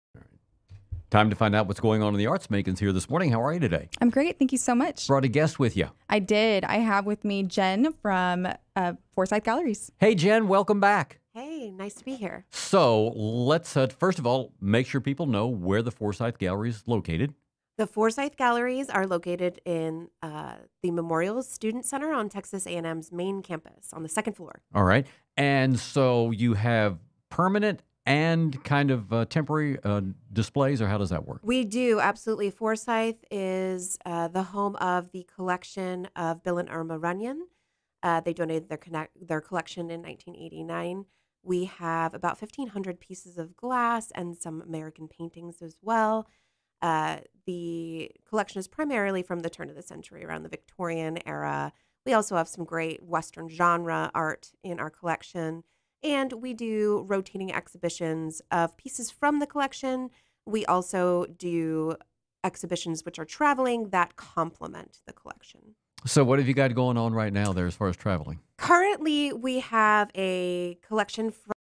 Forsyth Gallery - WTAW Interview 5/24/23
Check out an interview with Megan Le, ACBV Development and Operations Coordinator, and Jen Korolenko with the Forsyth Gallery on WTAW. Learn more about current shows and events with the Forsyth Gallery